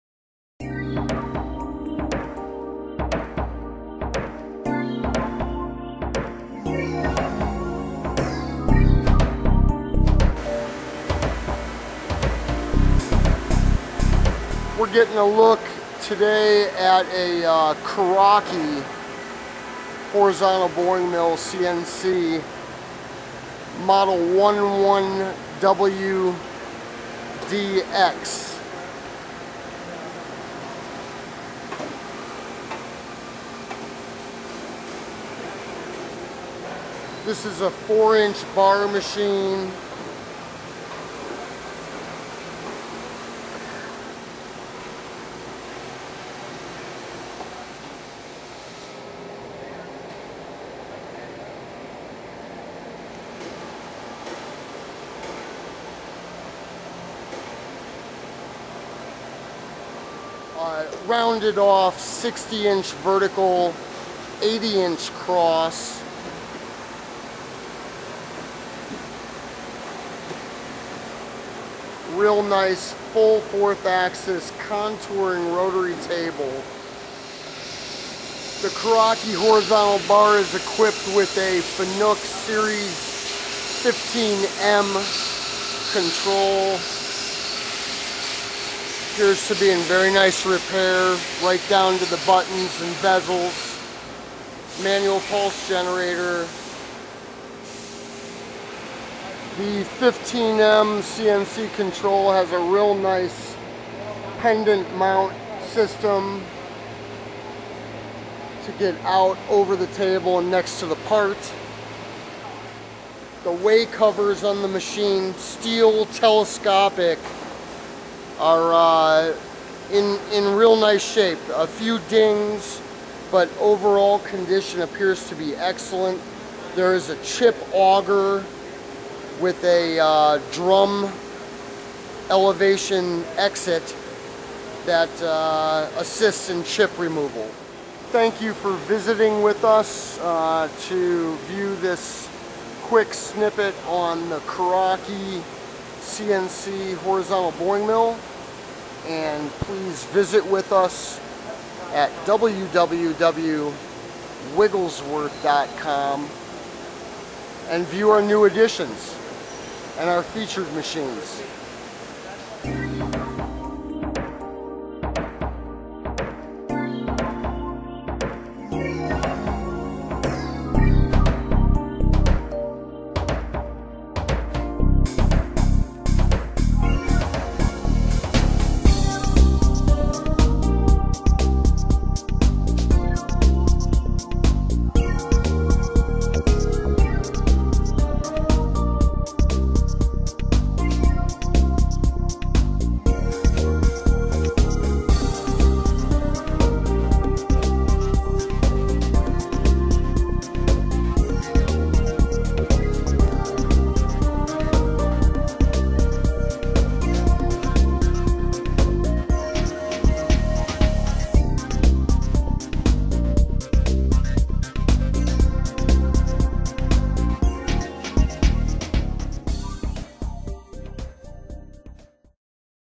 4" KURAKI KBT-11WDX CNC HORIZONTAL BORING MILL
ONE (1) USED
                       4" KURAKI CNC HORIZONTAL BORING MILL

 MODEL: KBT-11WDX
 NEW: 1995

 SPECIFICATIONS:

X-TRAVEL. 78.74"
Y-TRAVEL. 59.06"
W-TRAVEL. 51.18" (SADDLE)
Z-TRAVEL. 21" (SPINDLE)
SPINDLE DIAMETER. 4.5"
TAPER IN SPINDLE. CAT 50 W/ PDB
SPINDLE SPEEDS. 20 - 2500 RPM
TABLE CAPACITY. 14,000 LBS

 EQUIPPED WITH:

 FANUC 15-M CNC CONTROL W/ LARGE MEMORY CAPACITY
 FULL FOURTH AXIS ON TABLE
 WAY COVERS
 EXTERNAL COOLANT SYSTEM

 SPECIFICATIONS SUBJECT TO VERIFICATION